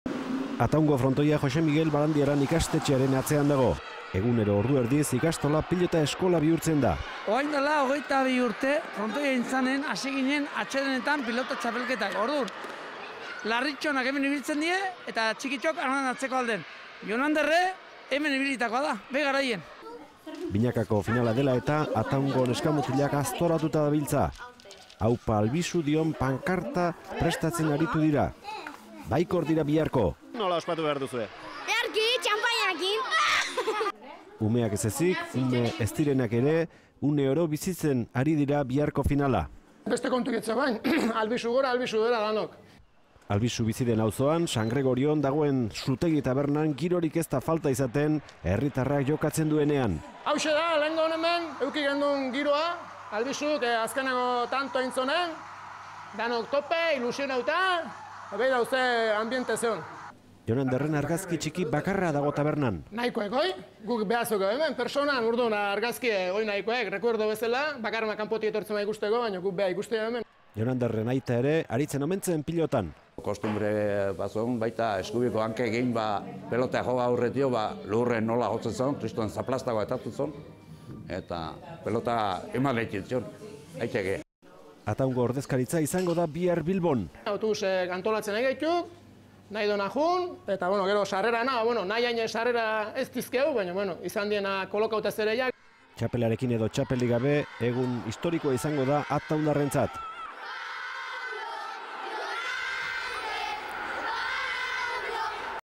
Ataun, pilotaren gailurrean Jon Ander Albisuri esker
Binakako Txapelketako finalaren atarian Albisuren herrian, Ataunen, baikor daude. Herritik autobusak antolatu dituzte Bilboko final handia zuzenean ikusteko, eta, ahal bada, txapela etxera eramateko.